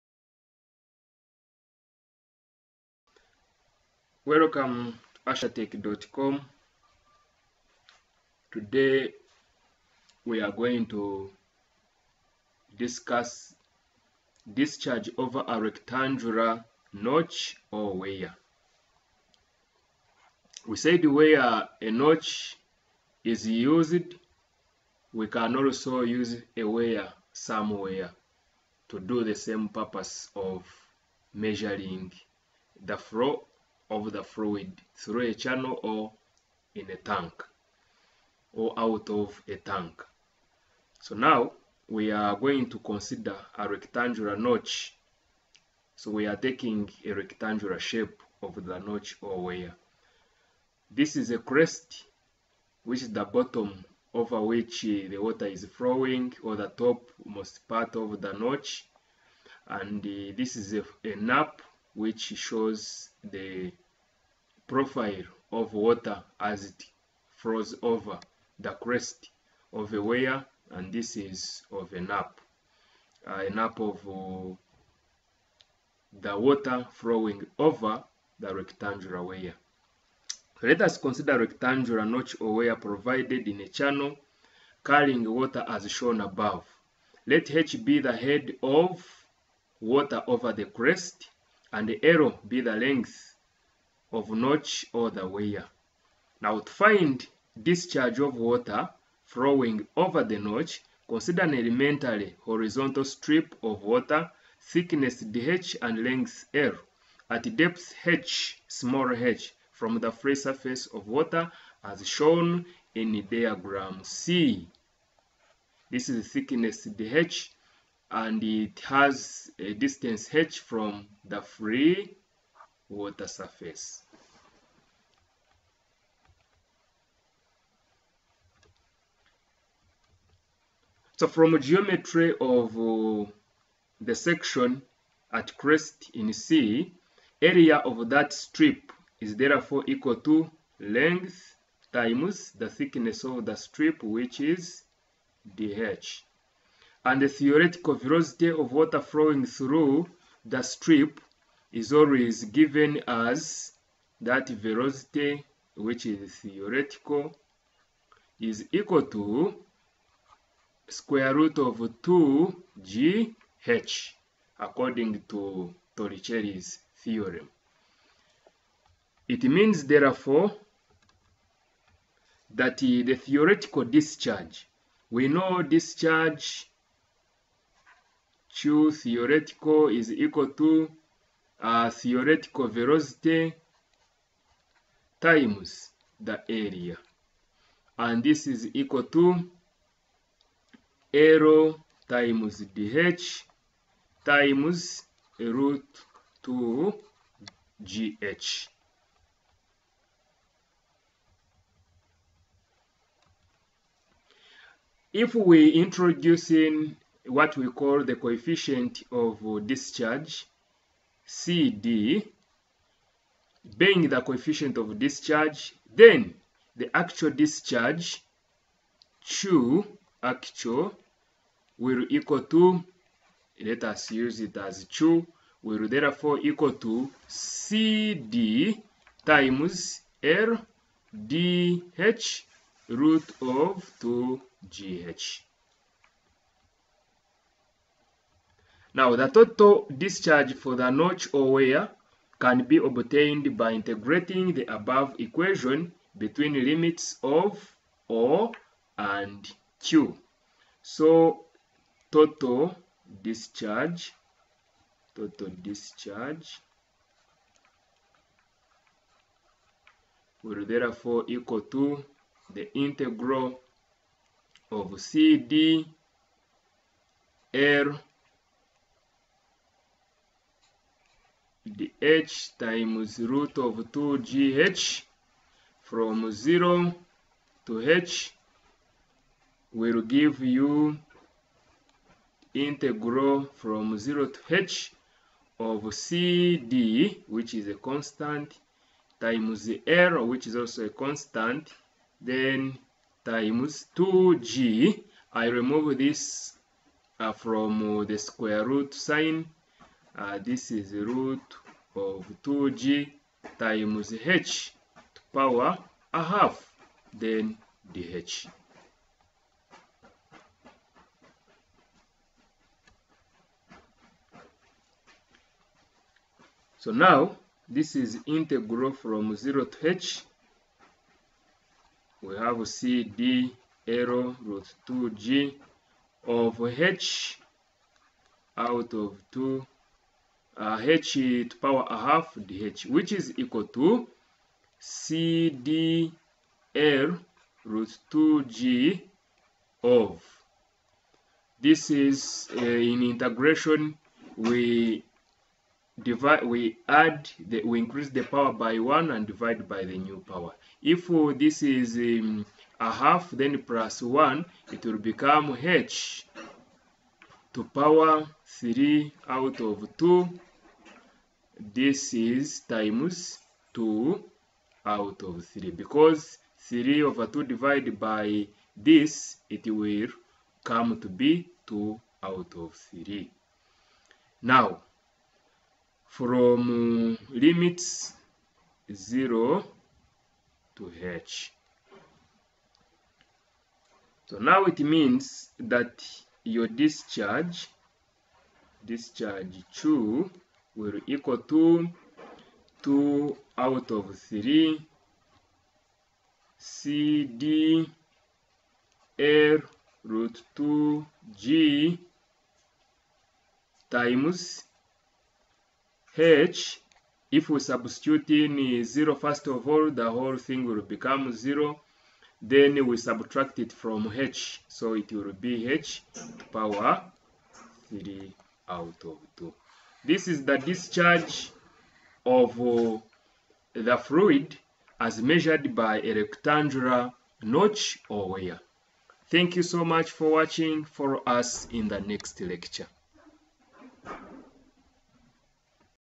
Discharge over a rectangular notch or weir | Fluid mechanics by Martin Ashaba
This video discussed the derivation of the formula for calculating discharge of a liquid flowing over a rectangular notch or weir..

it's fluid mechanics